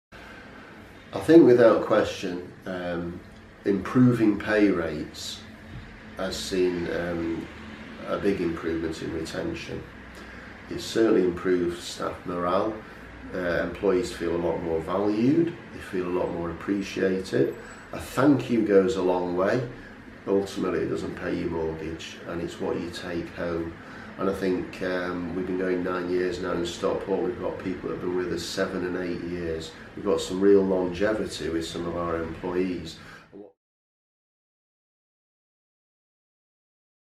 National Living Wage - Home Instead Stockport on improving pay rates
The Government's new National Living Wage becomes law in April. Make sure your business is ready. Visit livingwage.gov.uk for details.